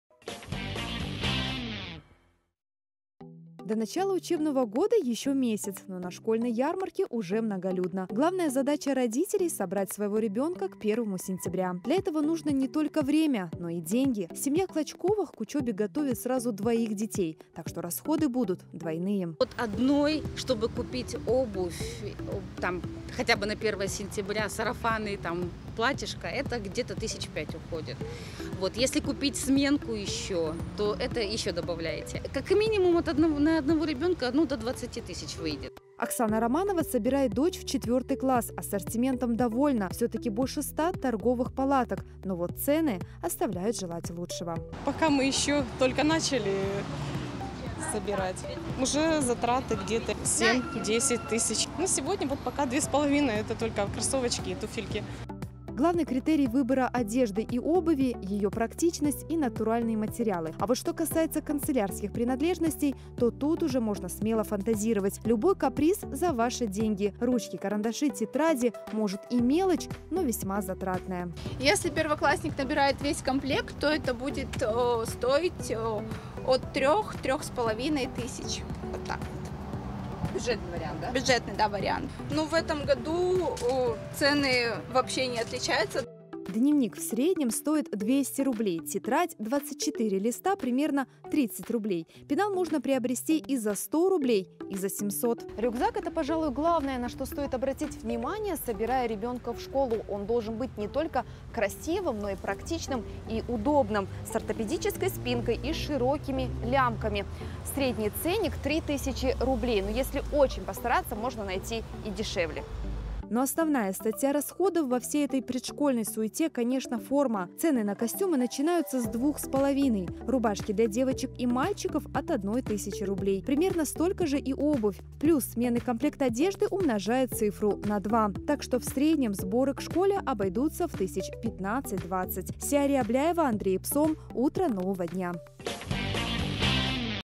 Как собрать ребёнка в школу и сколько это стоит?
Готовь сани летом, а телегу зимой. Последний месяц лета - пора задуматься об осени и начале учебного года. Как собрать ребёнка в школу и сколько это стоит - в сюжете Сеяре Абляевой.